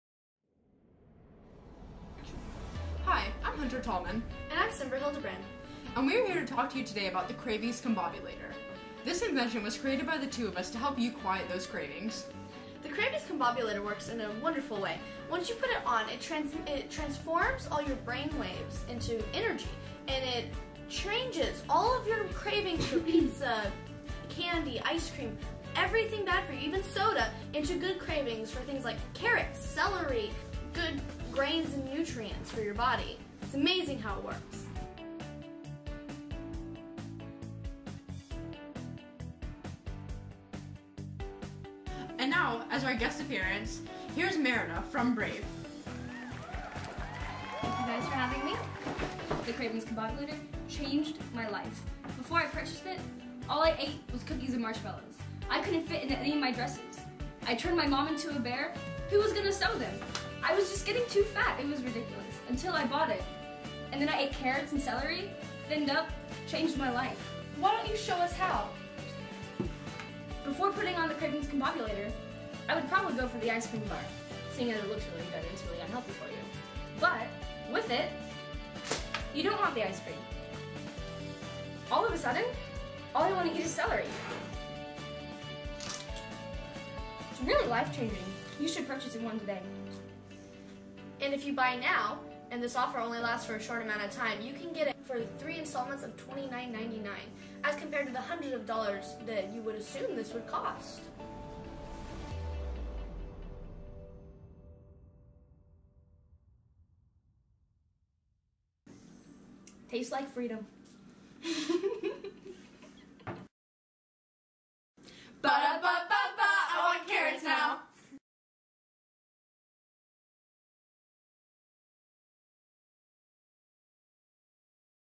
Cravings Combobulator commercial
For verbal communications class 14'. Hunter, Symbra, and Emili.